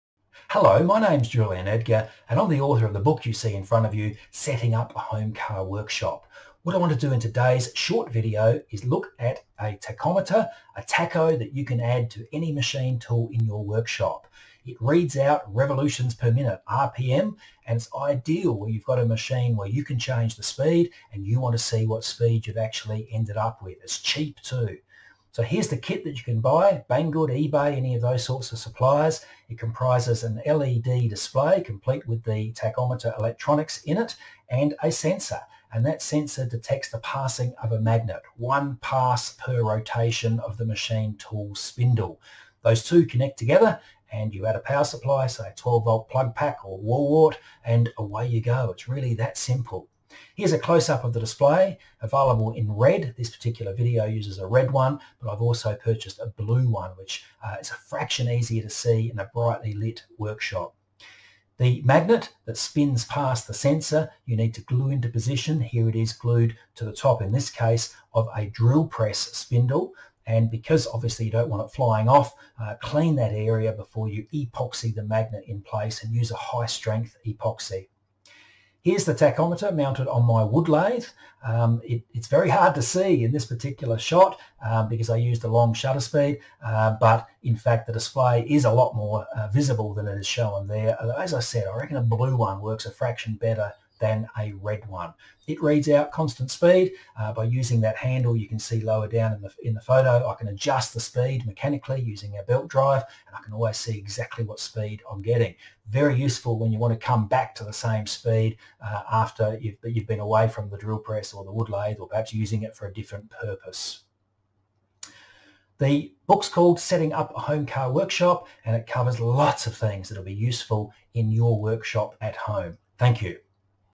Install your own machine tool tachometer
Cheap, easy and effective - see the speed of any machine tool. Ideal for use with adjustable speed tools like lathes and mills.